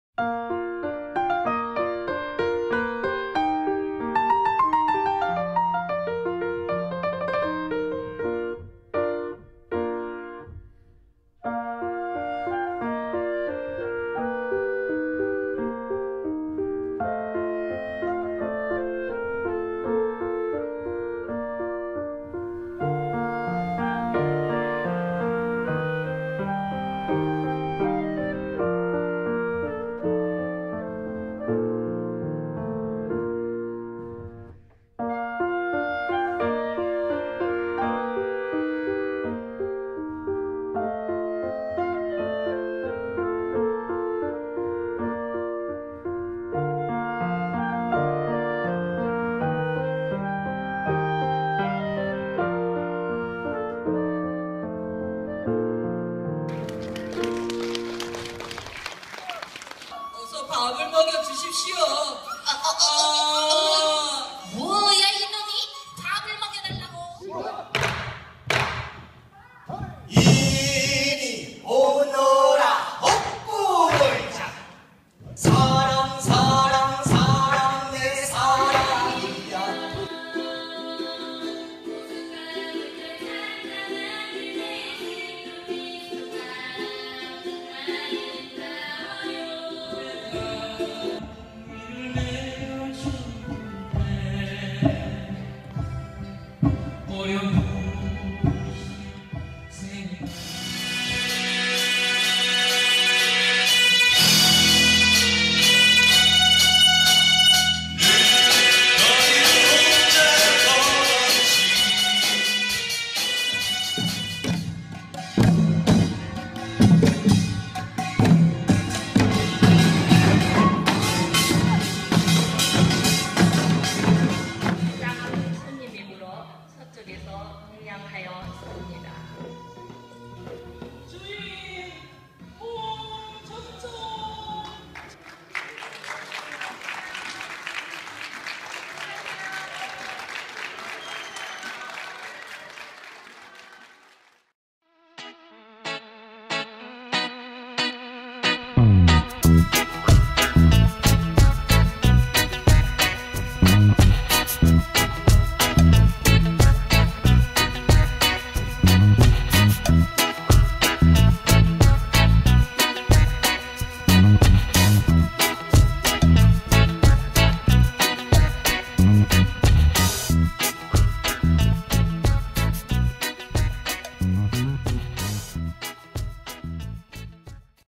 성남시 학습동아리 스스로축제(2013.11.15)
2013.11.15 성남시청에서 열린 평생학습동아리의 스스로 축제 현장.